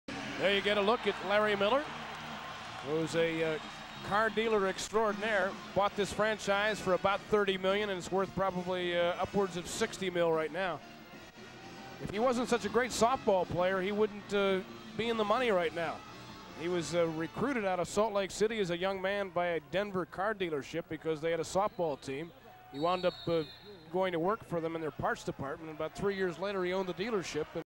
Larry H Miller 1989
LHM during a jazz pistons game in 1989